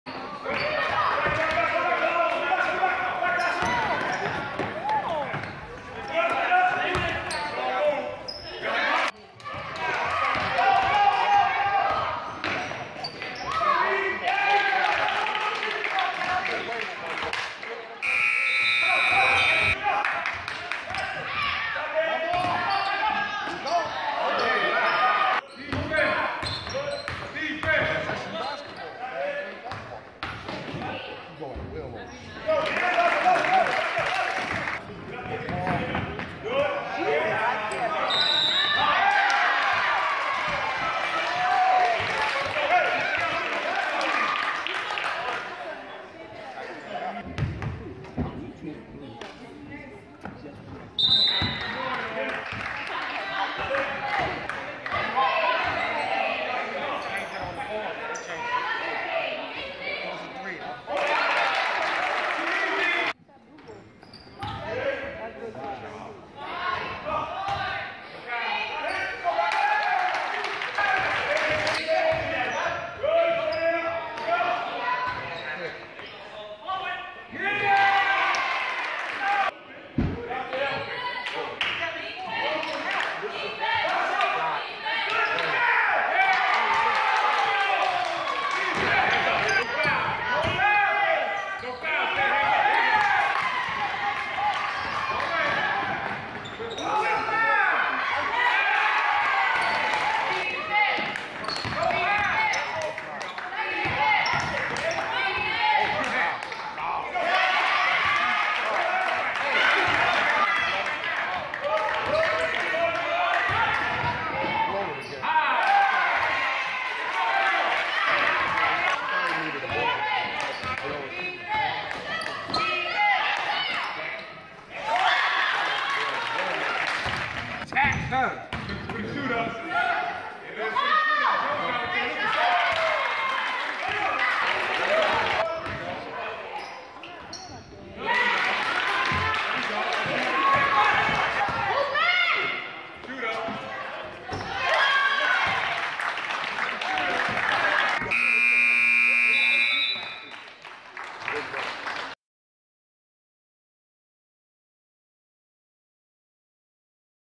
Lab Museum 61 Bryant 47
Highlights of Lab Museum United's 61-47 win over William C. Bryant in PSAL girls basketball action in Manhattan on Thursday night. Delilah Kalle had 20 points and Dasha Goodman had 19 points for Lab Museum. Brianna Randolph had 16 points and Anyla Harper had 14 points for Bryant. Check out the full game summary and box score